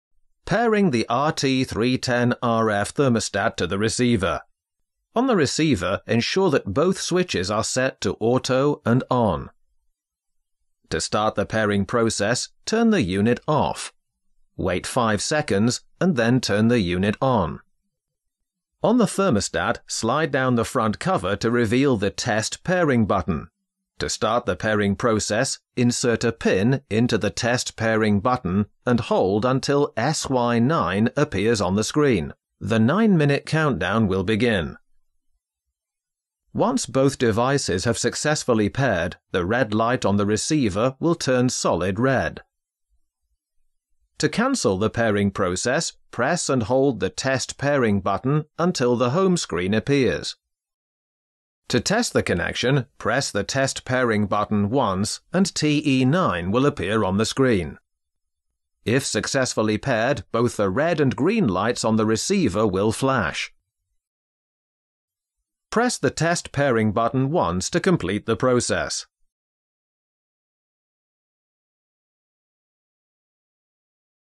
Salus RT310RF Διαδικασία ζευγοποίησης - Pairing process
Αναλυτικές Οδηγίες:
1) Αποσυνδέστε εντελώς τον δέκτη RXRT510 από την τροφοδοσία (αποσυνδέστε το καλώδιο) + βγάλτε τις μπαταρίες από τον θερμοστάτη.
2) Περιμένετε 10 λεπτά (hard reset) – (εμπειρικά έχουμε παρατηρήσει ότι η διαδικασία γίνεται και με απλό reset – με απλή διακοπή παροχής ρεύματος – χωρίς να περιμένετε 10 λεπτά.)
3) Πάρτε το δέκτη RXRT510 και ρυθμίστε και τους δύο διακόπτες σε ON και AUTO. Συνδέστε το στο τροφοδοτικό.
4) Το RXRT510 αναβοσβήνει 3x ΚΟΚΚΙΝΟ και στη συνέχεια 2X ΠΡΑΣΙΝΟ (δηλαδή εμφανίζει την έκδοση firmware που είναι 3.2). Μετά από αυτό, χρησιμοποιήστε τον επάνω διακόπτη και μεταβείτε γρήγορα στο OFF και πάλι στο ON (κάντε το αμέσως μόλις σβήσει το πράσινο LED). Απλώς μεταβείτε στο OFF και επιστρέψτε στο ON. Μπορείτε να το κάνετε ακόμη και δύο φορές για να βεβαιωθείτε (εντός 2 δευτερολέπτων).
5) Ο Δέκτης RXRT510 θα πρέπει να αρχίσει να αναβοσβήνει με κόκκινο LED (αν δεν το κάνει, ξαναδοκιμάστε τα βήματα 1-4).
6) Τοποθετήστε τις μπαταρίες πίσω στο θερμοστάτη. Περιμένετε μέχρι να ενεργοποιηθεί.
7) Κρατήστε πατημένο το κουμπί TEST/PAIRING για 3 δευτερόλεπτα μέχρι να δείτε το "SY9".
8) Περιμένετε έως ότου η δίοδος RED στο δέκτη σταματήσει να αναβοσβήνει (μπορεί να χρειαστεί ως ένα λεπτό μέγιστο)
9) Κρατήστε ξανά το κουμπί TEST/PAIRING για 3 δευτερόλεπτα μέχρι να δείτε την κύρια οθόνη στον θερμοστάτη (θερμοκρασία).

Ένας χρήσιμος οδηγός για τον συγχρονισμό του θερμοστάτη και του δέκτη Salus RT310RF.
Ο δέκτης χρησιμοποιείται για τη σύνδεση των καλωδιώσεων και την ενεργοποίηση/απενεργοποίηση της θέρμανσης και ο πομπός αποτελεί τον ασύρματο θερμοστάτη του συστήματος.